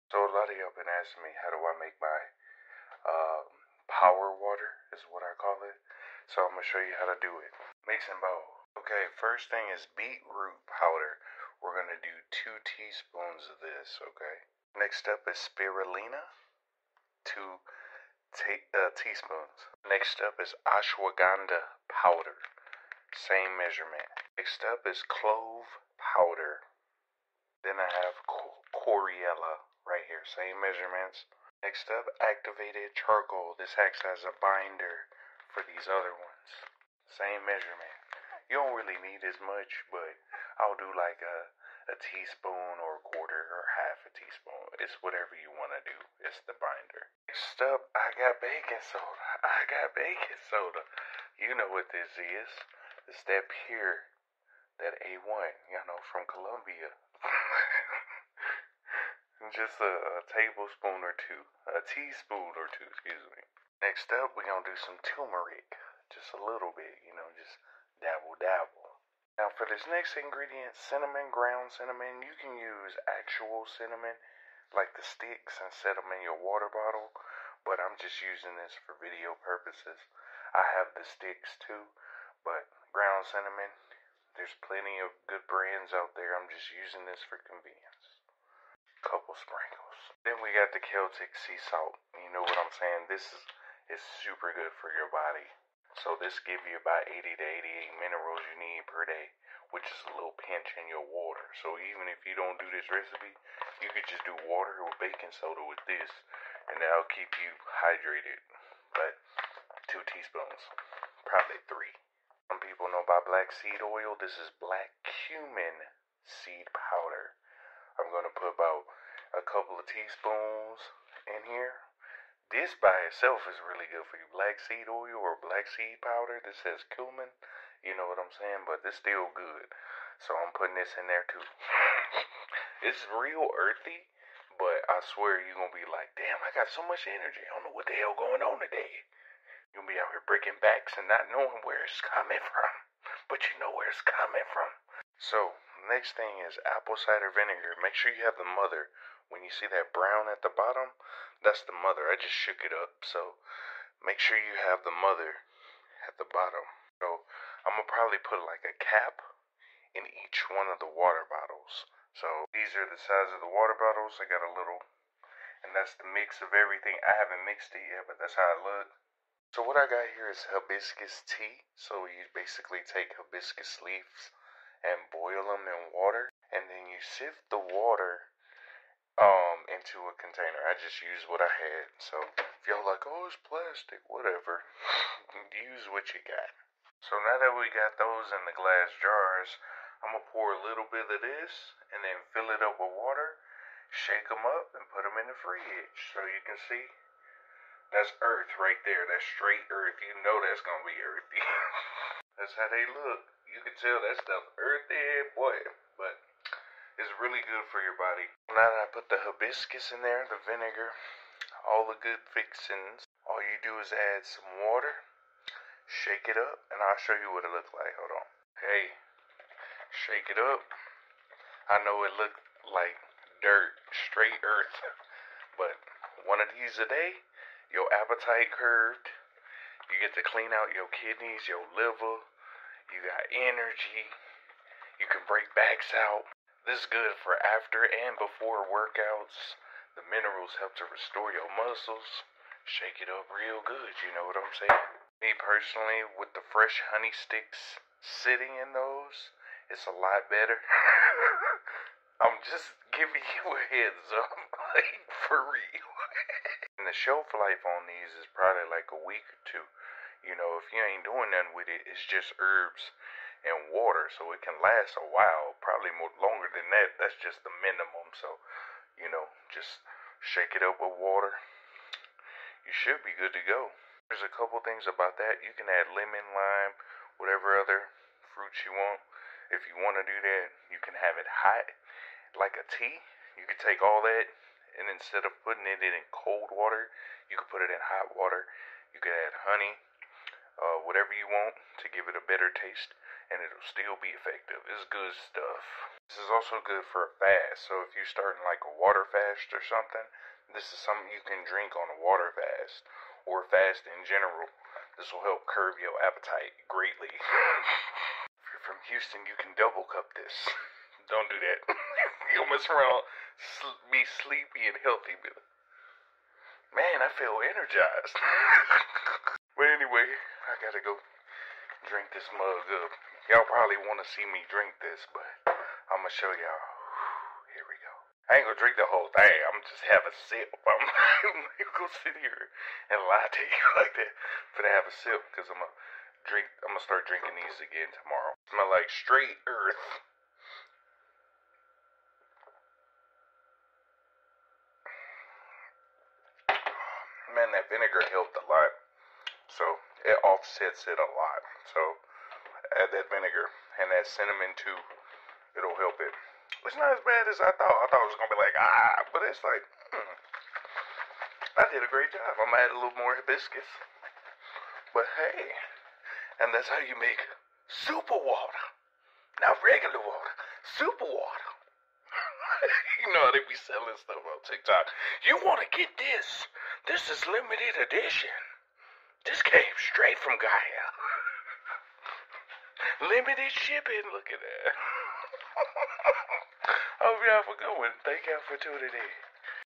Superpower water recipe!
Here is the recipe for the water:

(You can adjust measurements to your taste)

Beet Root Powder
Spiralina
Ashwahganda Powder
Clove powder
Chlorella
Activated charcoal
Baking soda
Tumeric
Cinnamon (Ground or sticks)
Celtic Sea Salt
Black Cumin Powder
Apple Cider Vinegar (With Mother)
Hibiscus Tea

You can add lemon or fruit, have it hot or cold, and it's good for a fast.

This is super great for your body!